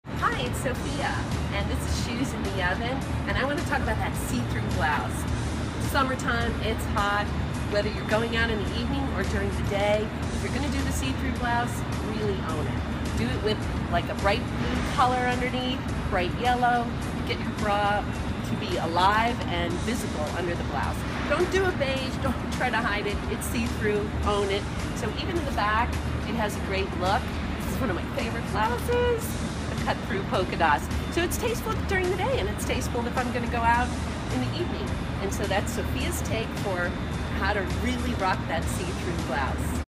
Sophia's Take Fashion: Rock The See Thru Blouse
That's the only way to wear it!
Don't try to hide the fact that it's see thru, own it... Change it up underneath the blouse with a pretty blue bra, or the bright yellow one that you rarely wear. Here's the perfect excuse to show it off! Another option is a colored tank!